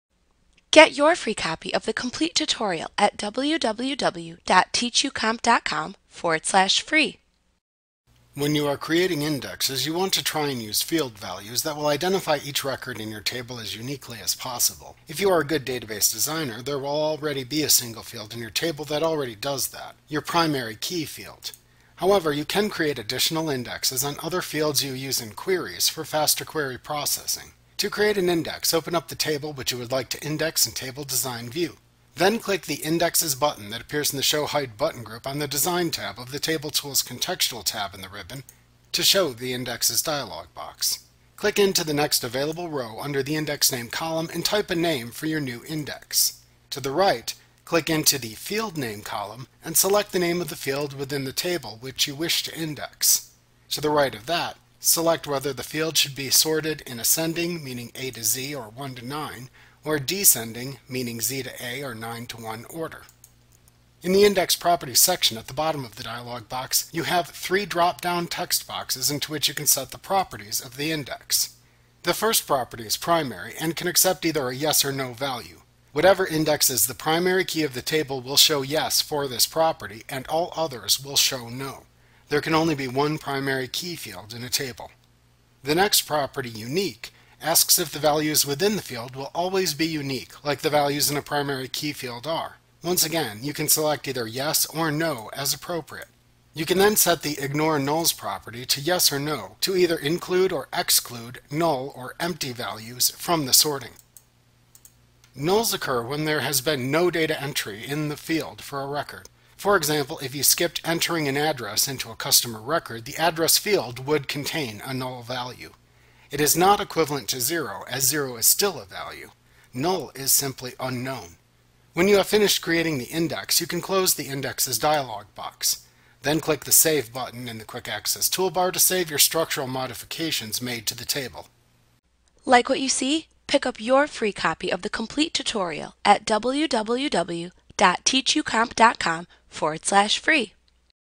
Access 2016 Tutorial Creating Indexes Microsoft Training
Learn about creating indexes in Microsoft Access - the most comprehensive Access tutorial available. Visit us today!